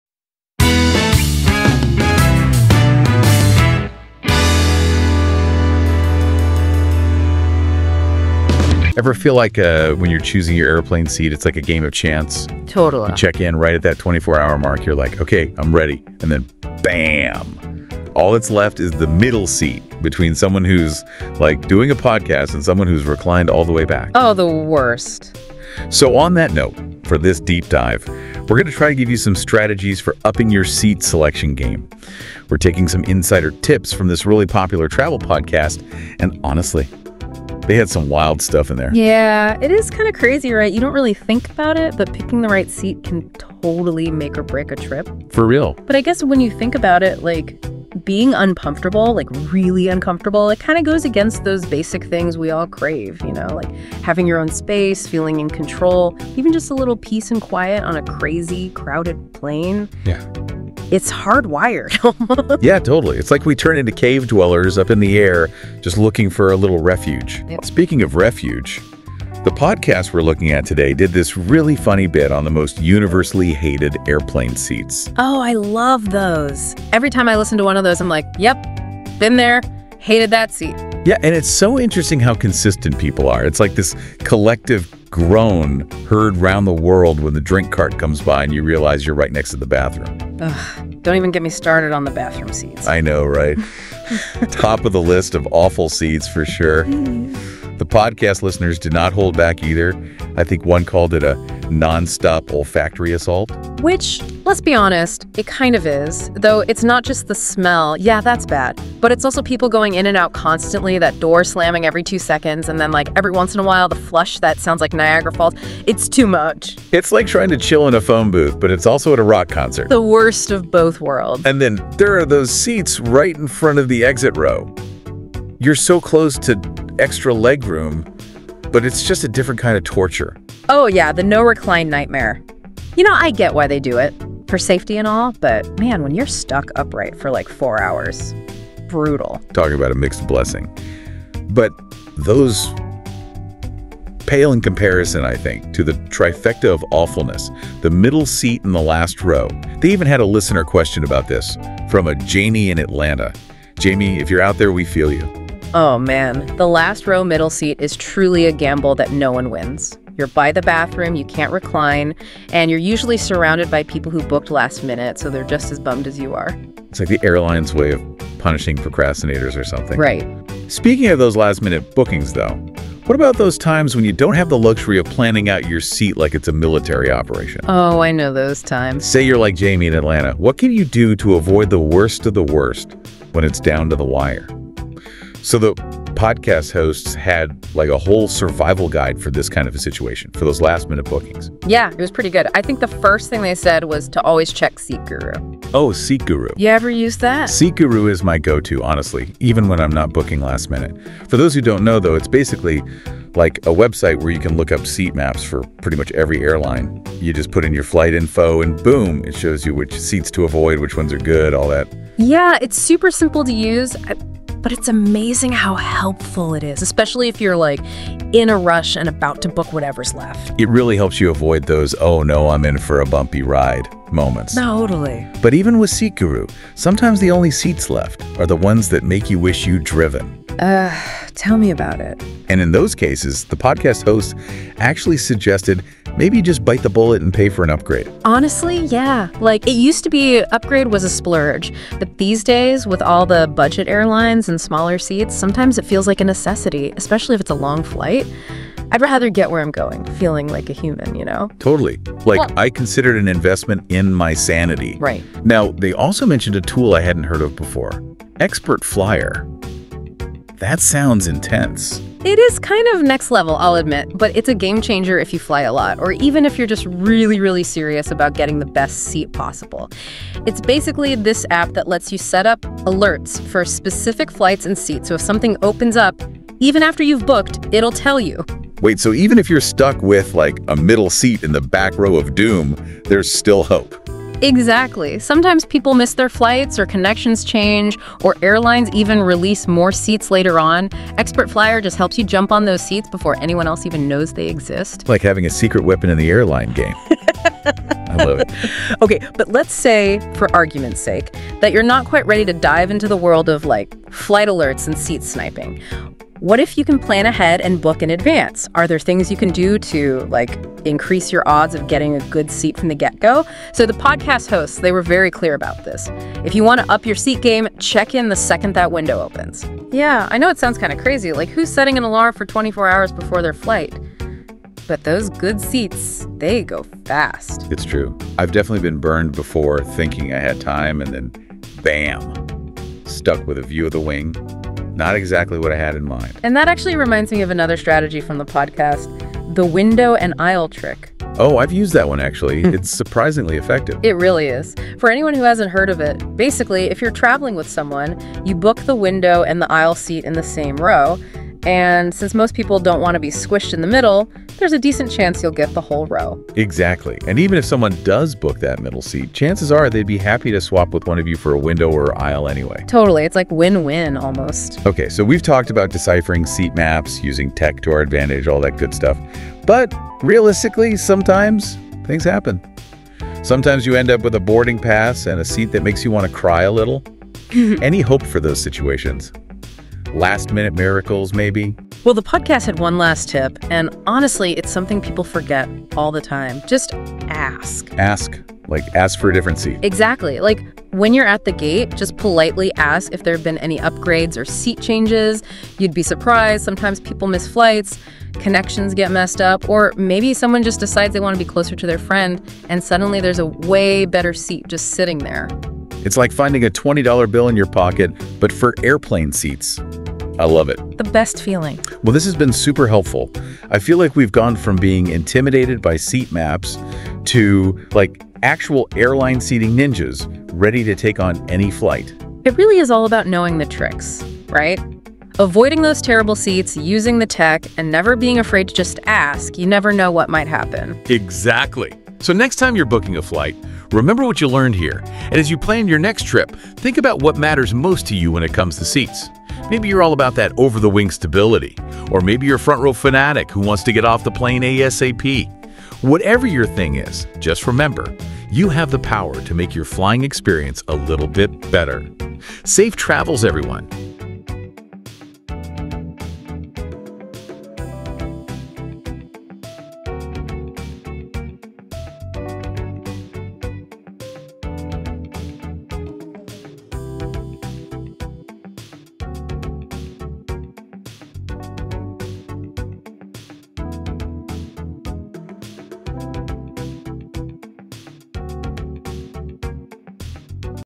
The WORST Airplane Seats! How to AVOID Them for COMFY Travel
Are you tired of being cramped in uncomfortable airplane seats during your travels? In this video, we reveal the absolute WORST airplane seats to avoid for a more enjoyable flying experience! Learn about the specific seat locations that can ruin your trip and discover tips on how to secure comfy seating for your next flight. From legroom issues to proximity to the lavatory, we cover it all! Whether you’re a frequent flyer or planning your next vacation, this guide will help you make informed choices when booking your flight. Don't let bad seats ruin your travel plans—watch now and travel smarter! ✈️